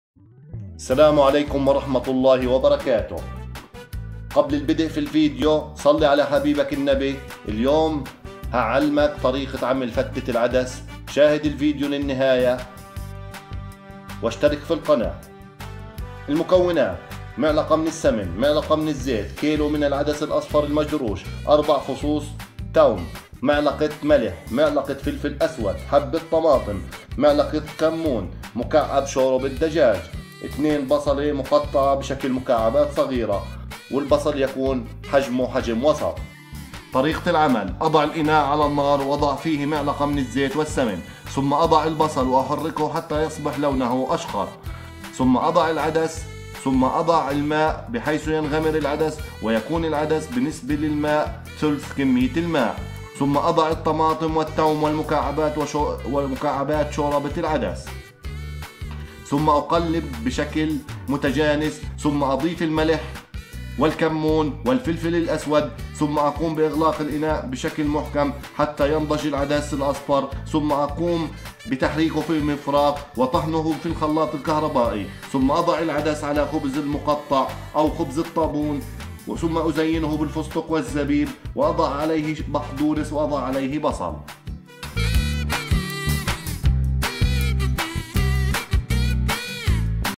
فتة العدس الاصفر ضاع عمرنا مع العدس شاهد بنفسك 2021
طريقة عمل فتة العدس  بسهولة وسرعة عالية فته العدس الجميلة المفيدة للجسم  في هذا الفيديو القصير سنتعلم  فته شوربه العدس بالفستق والبصل والزبيب وهذه هي فتة العدس على الطريقة الفلسطنية
لا تنسوا الإشتراك في قناتنا وتفعيل زر الجرس "🔔"